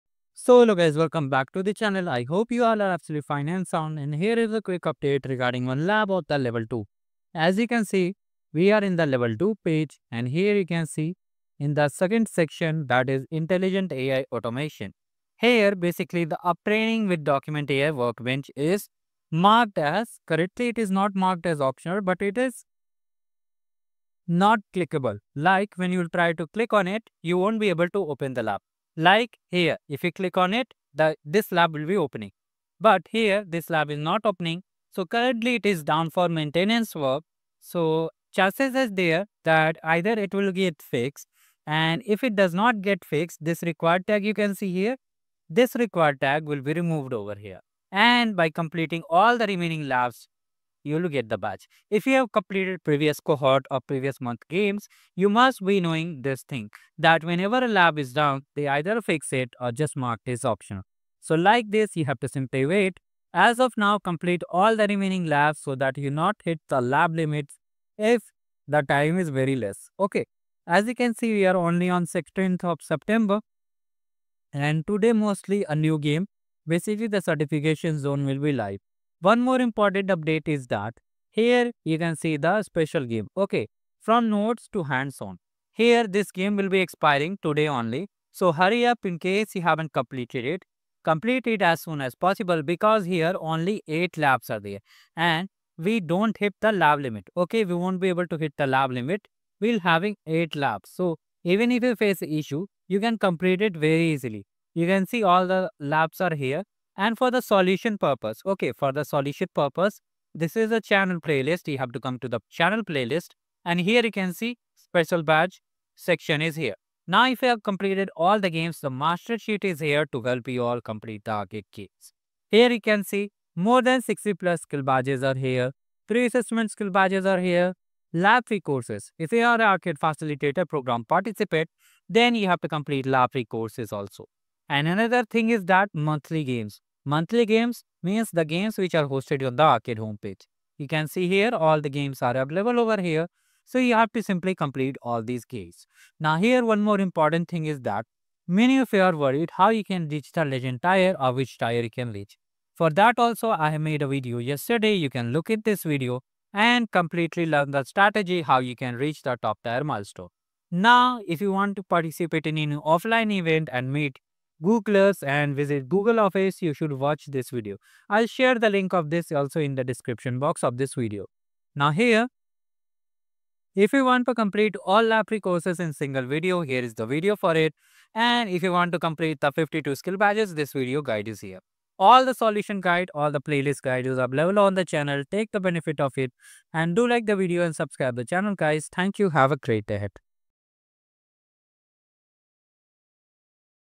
GoogleArcade September Labs Update | Earn 52 Google Skill Badges in 4 Day |Resource Guide
Can we Reach the Google Arcade Legend Milestone | A Step-by-Step Guide to Free Swag & Goodies (2025)

🔍 Common Queries
qwiklabs
arcade facilitator